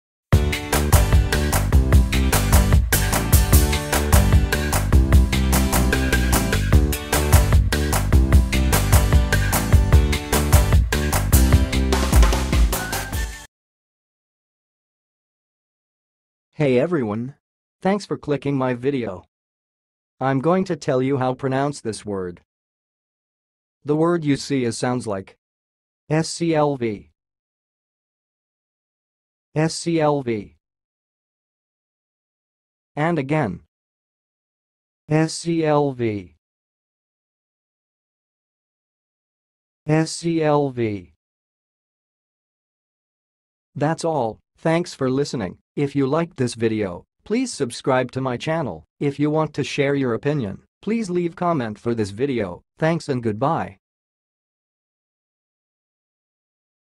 SCLV - HOW TO PRONOUNCE IT!?
Watch how to say and pronounce "sclv"!